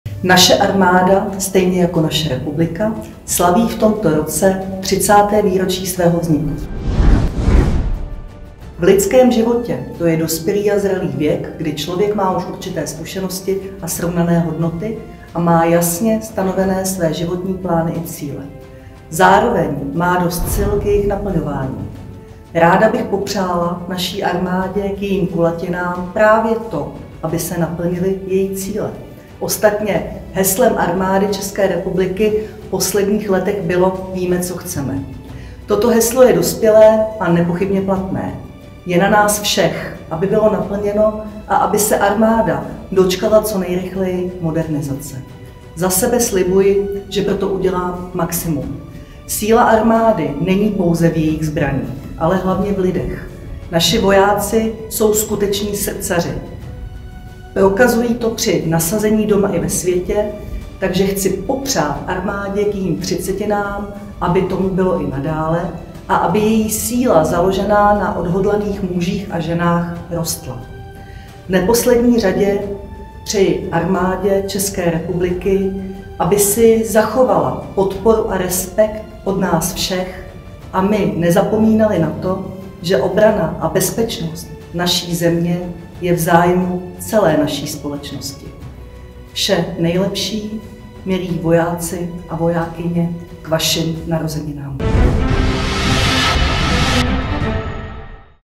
Přání ministryně obrany k 30 letům AČR
Před 30 lety vznikla Armáda České republiky.
„Naši vojáci jsou skuteční srdcaři. Chci popřát armádě k jejím třicetinám, aby tomu tak bylo i nadále a aby její síla založená na odhodlaných mužích a ženách rostla,“ poblahopřála armádě ministryně obrany Jana Černochová.